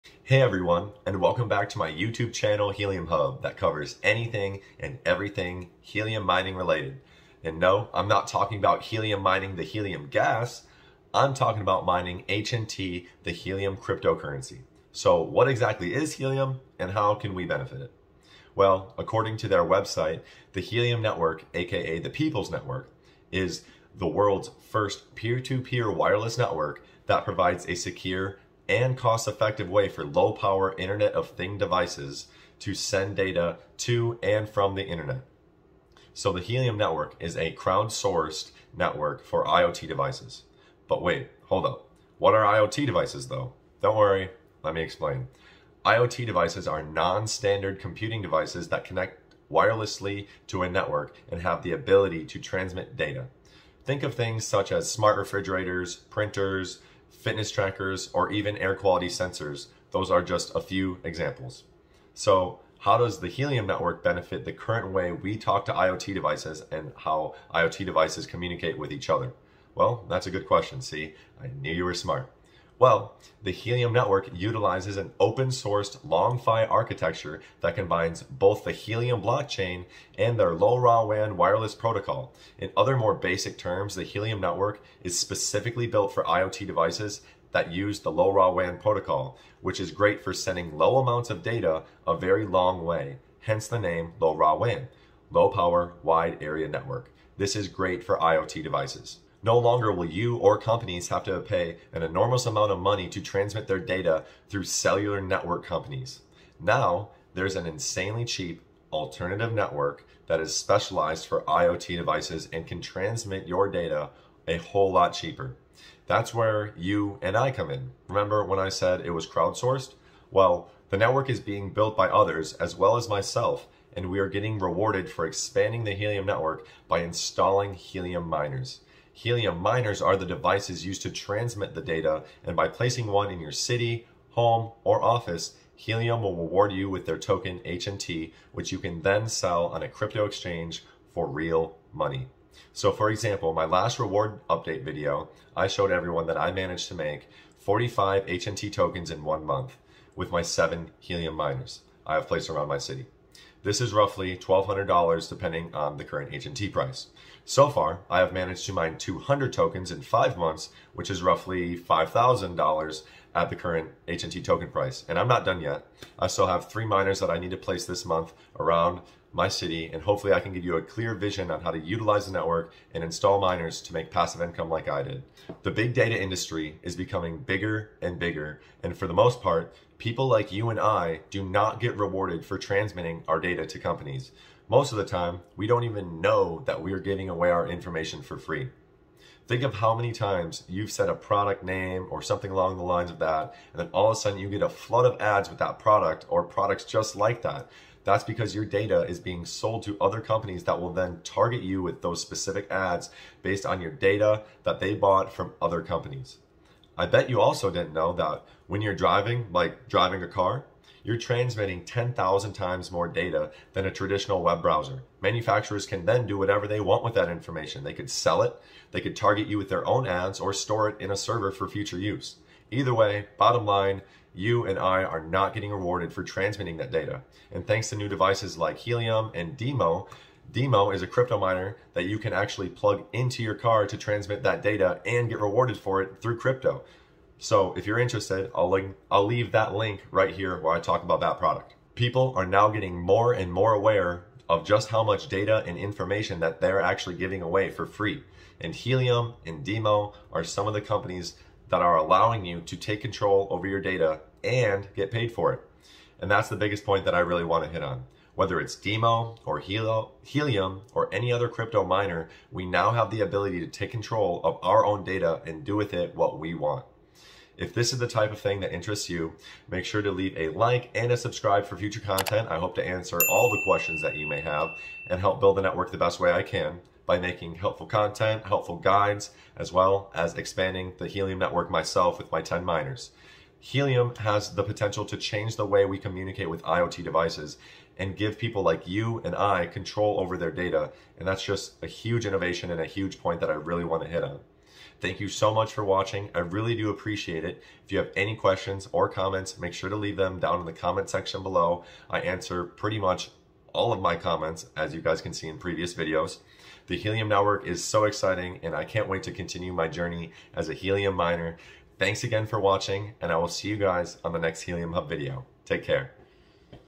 Helium Mining Explained Is It Still Worth It in 2022?
The Helium network has the ability to completely change the way we communicate with IOT (Internet of Things) devices. By creating a network that is specifically made for IOT devices, Helium has over 650k miners or "hotspots" and is rising at an exponential rate. I currently deploy 10 bobcat miners

This network is meant to lower the cost to use the network and increase the connectivity of IOT devices. Company's now use either cellular data or WIFI to connect their IOT devices to, this is not ideal. IOT devices only need to send a very small amount of data and send it a long distance. This is exactly what the helium network is doing. Its using the LoRaWan protocol as well as the helium block chain to make a dedicated network for IOT devices that specializes in sending low amounts of data, a very long way. This reduces the cost of connecting and sending data from IOT devices. By placing a miner in your home, office,  or city, you are expanding coverage for IOT devices to connect to. And for that, you can get rewarded HNT tokens which you can then sell on a crypto exchange for $$.

I currently have 10 bobcat miner setups and In other video I explain the profit and passive income that I have made with them so far. This video is intended for helium mining beginners.

Schedule Helium Consultation (EARN MORE REWARDS!)
Nowakit.com

Keywords helium mining helium mining explained helium mining setup